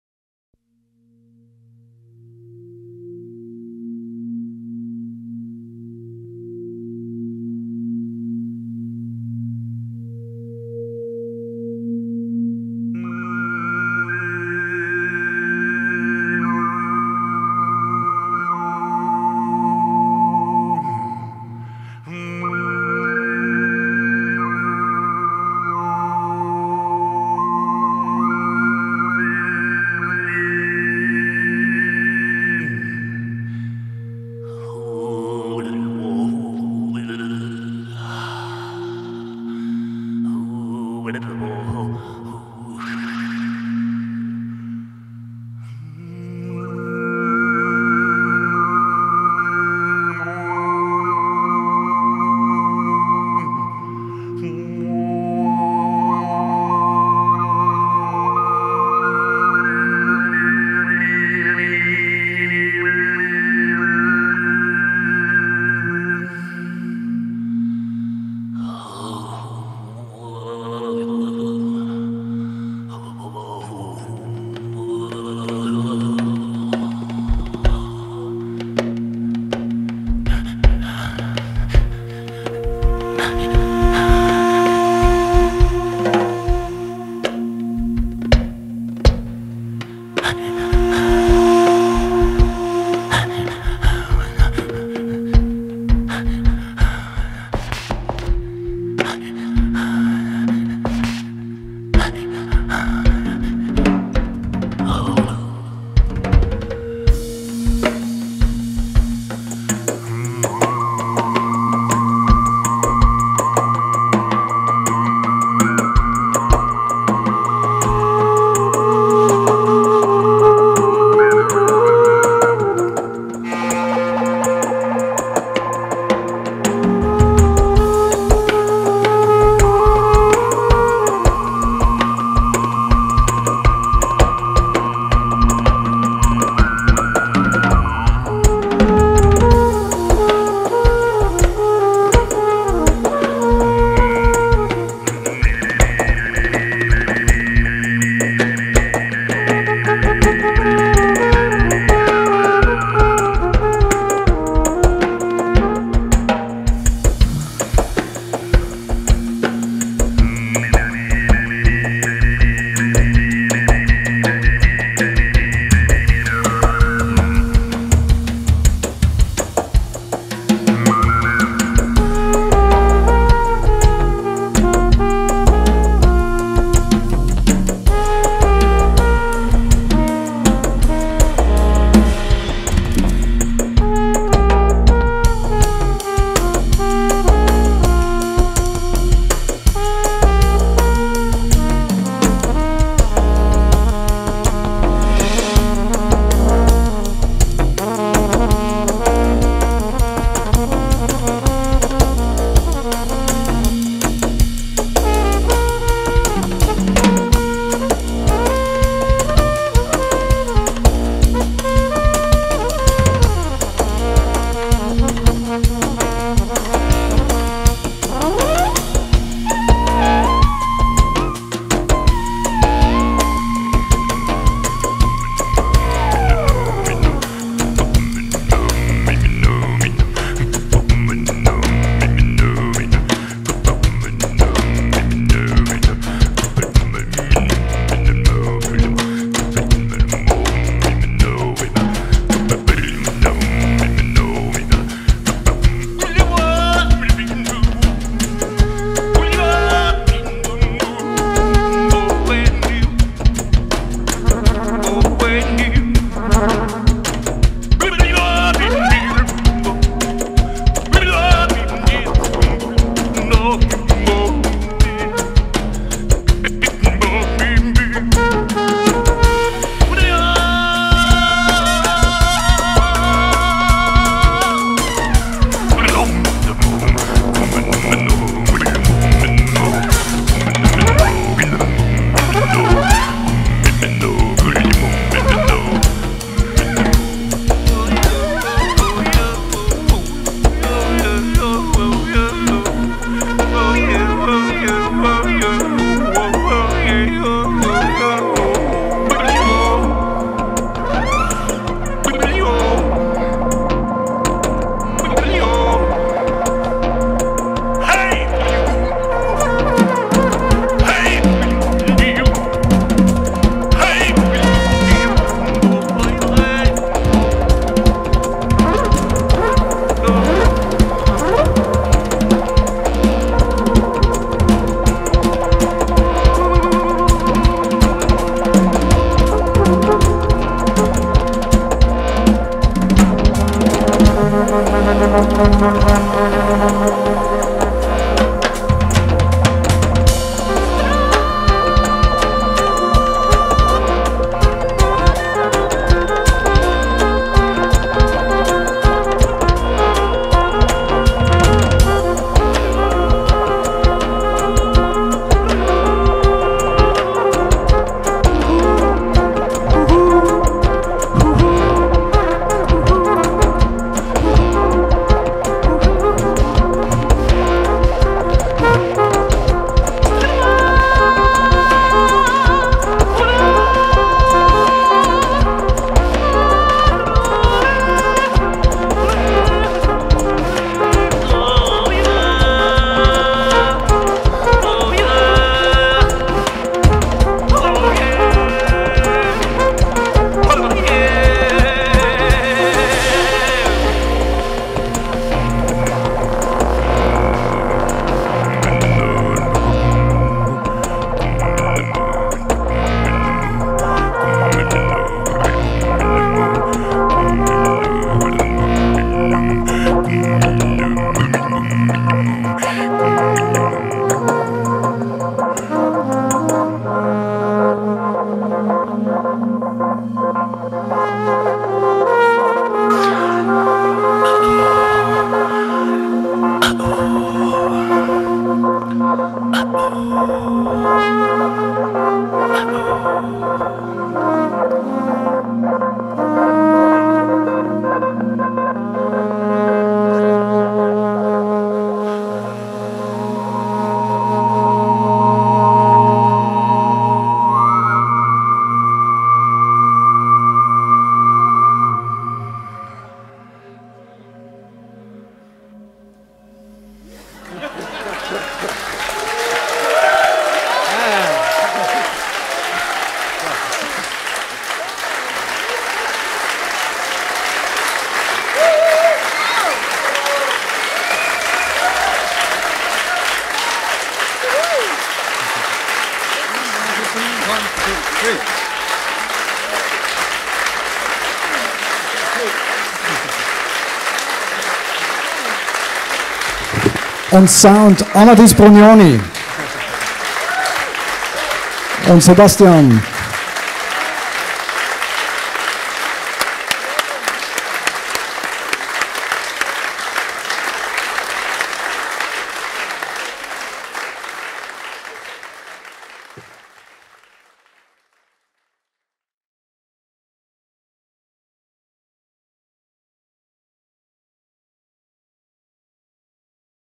BOOMLESS/gregor @ Beyeler Foundation invites Hildegunn Øiseth & Christian Zehnder - official
This was a spontaneous gathering at Fondation Beyeler for the Kulturpreis given
to Gregor Hilbe / Boomless.

Sound by Amadis Brugnoni